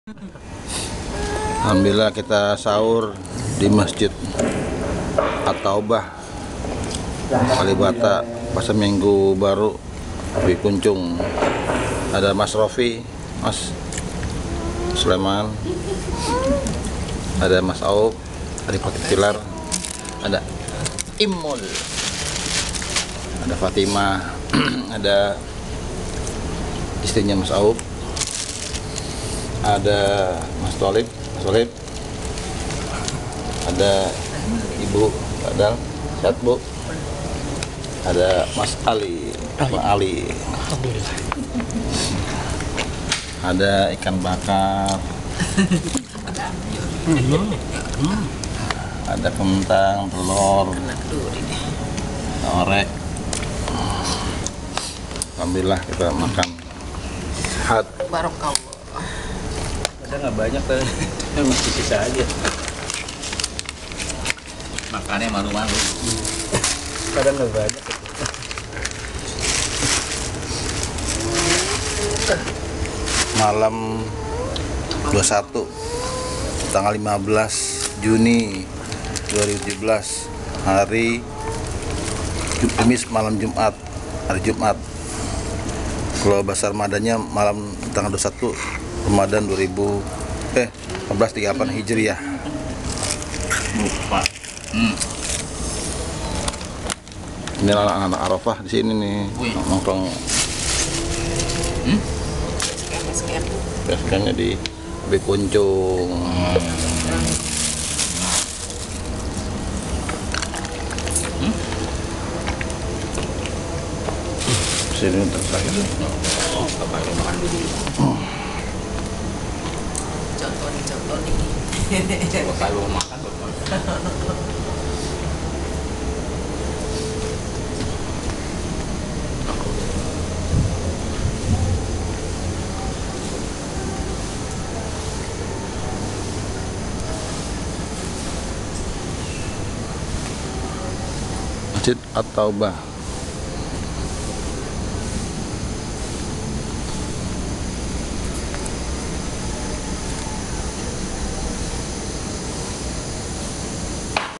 Itikaf malam 21 Ramadhan 2017
Malam 21 Ramadhan,Pendopo Agung MNA mengadakan acara itikaf bersama jamaahnya dan malam ini berkumpul di Masjid Habib Kuncung X-Bata,alhamdulillah,ketua Arafah ikut hadir bersama istri dan anaknya dan beliau sendiri pula bawa makanan untuk sahur nanti.(DSCN6499)